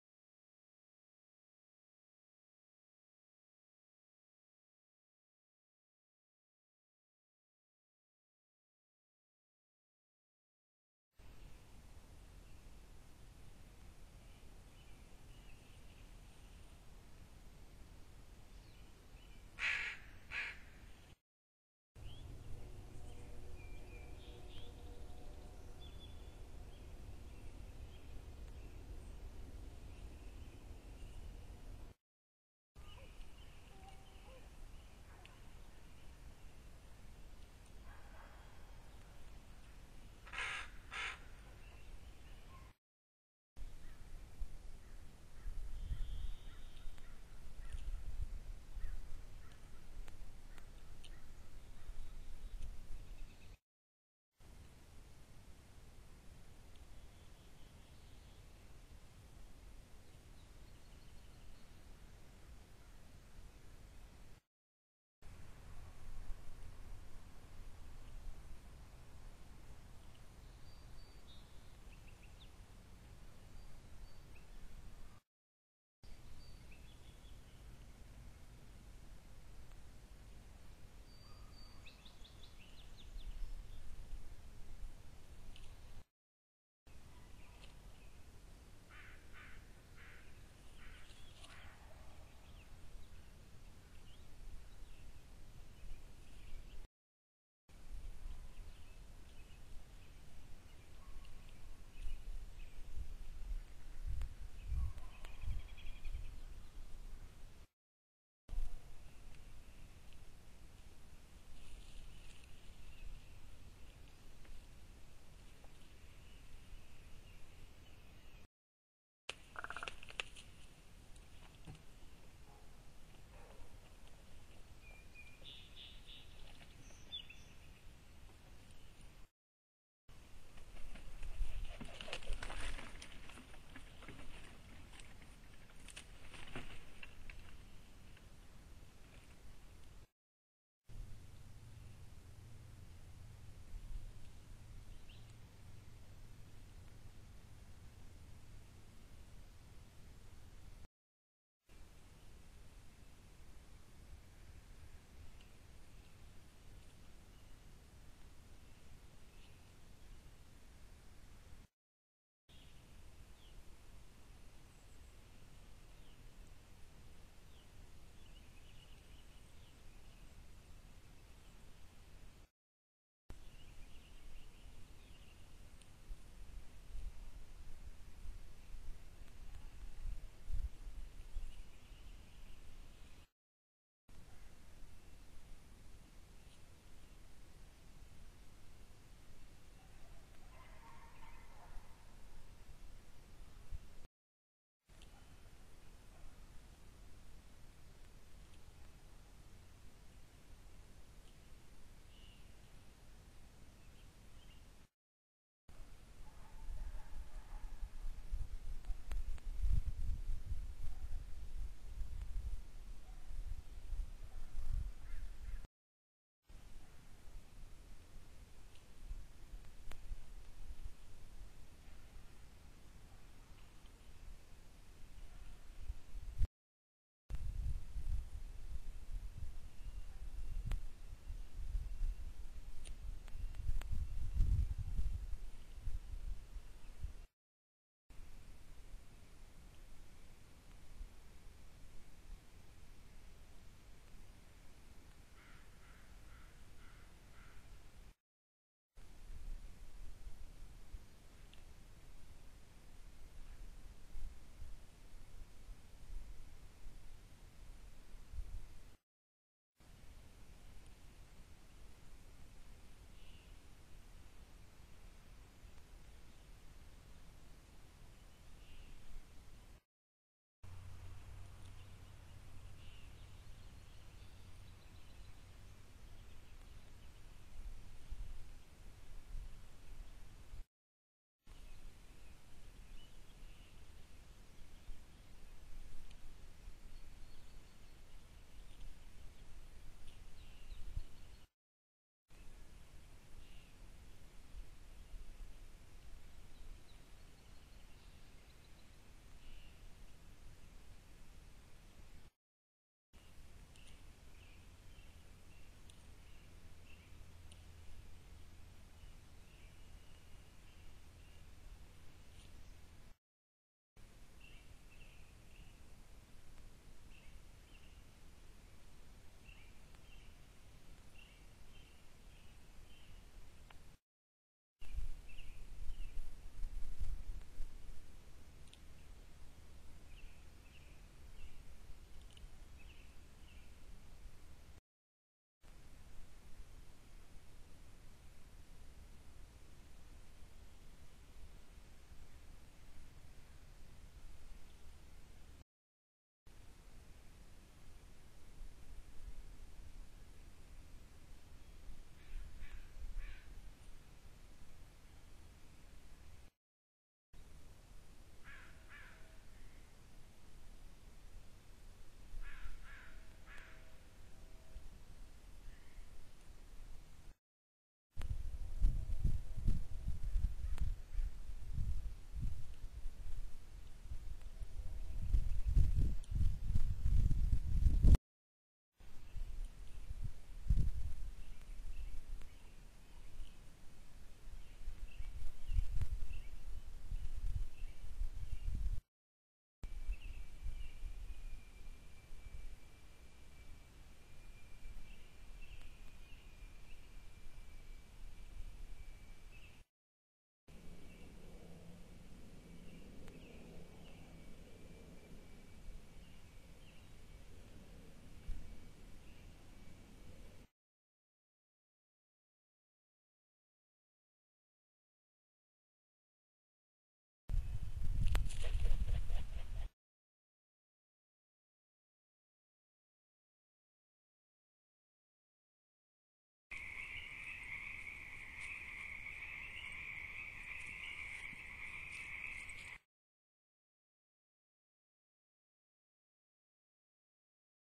201303 Eagles possum
We had a sad event, a possum passed away on our property the other day.  We decided to let mother nature take it course...
With a game cam posted on a tree near by.
This is all the video shots we got strung together. We were surprised by what came in.  We expeced a coyote or 2, but there were none.